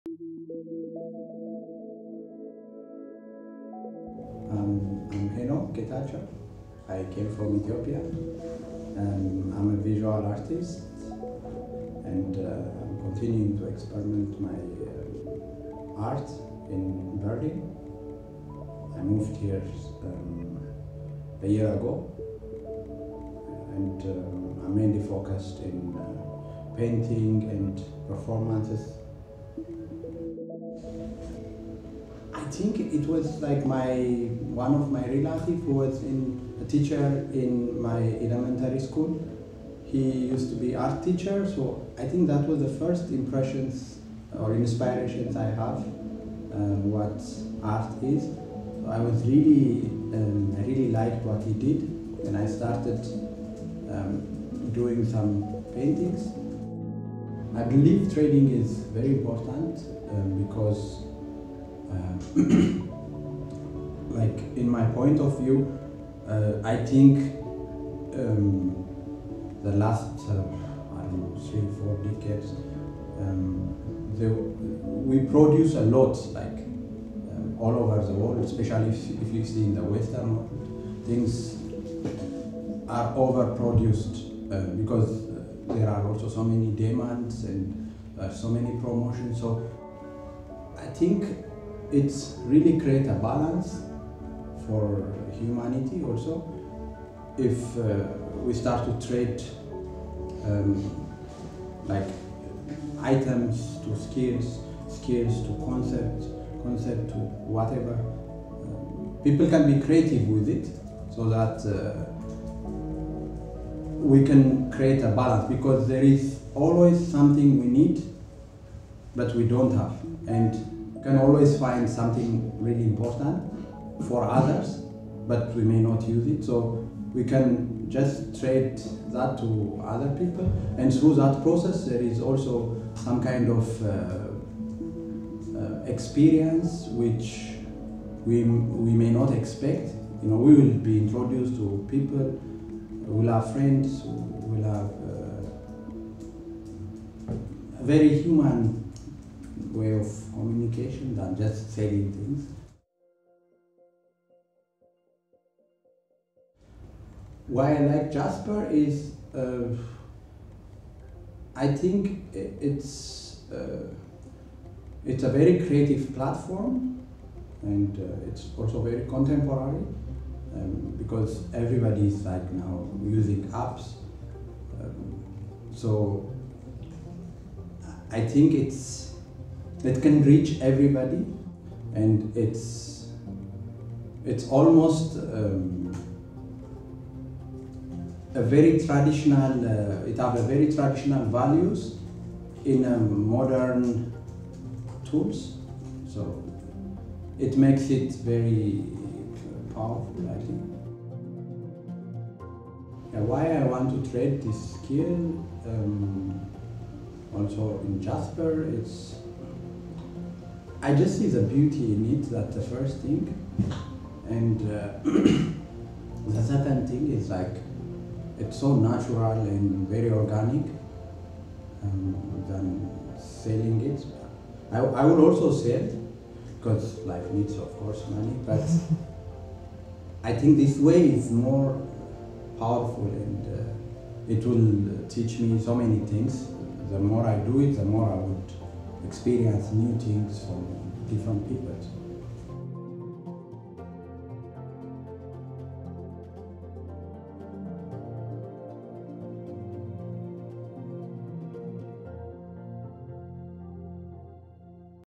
Henok - Jaspr Trader (Full Interview)
Meet Henok, an Ethiopian visual artist currently based in Berlin. He's trading art, art classes and more on Jaspr, and in this interview he shares his thoughts on overproduction and how trade can help people around the world.